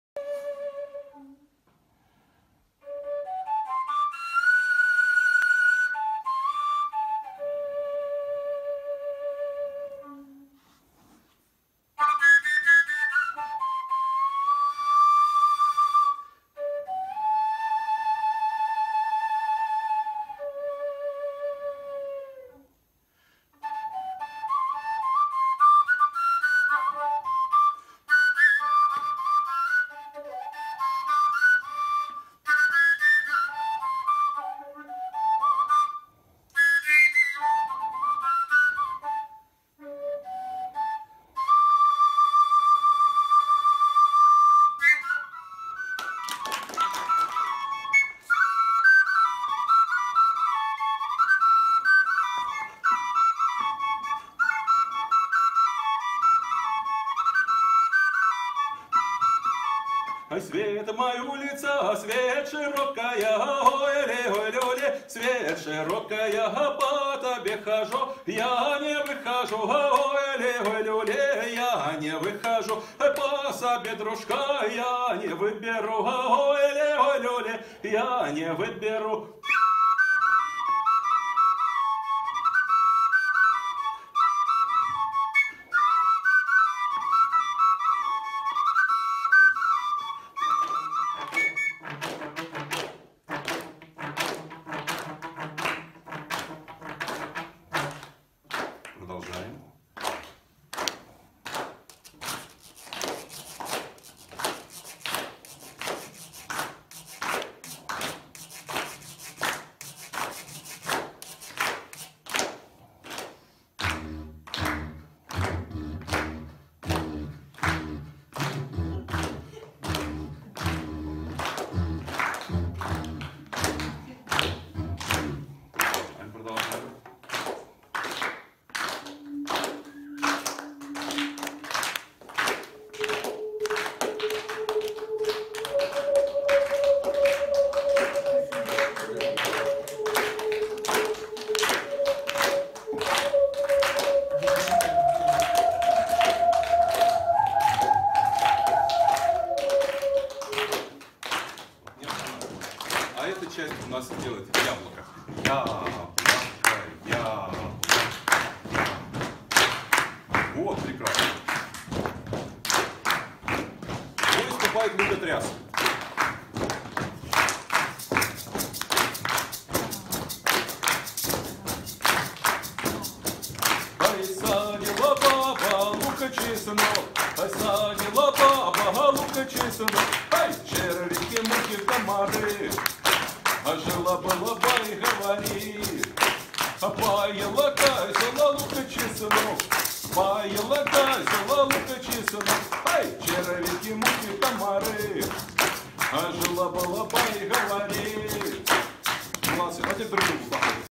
ЛОМОВ Р.В. - Выступление в Каменке
Компиляция эпизодов выступления мульти-инструменталиста Романа Владимировича Ломова после окончания фестиваля "Жар-птица", в Каменке, в Краеведческом музее.
23 сентября 2017,
Краеведческий музей,
г. Каменка,
съёмка моя, с IPhone7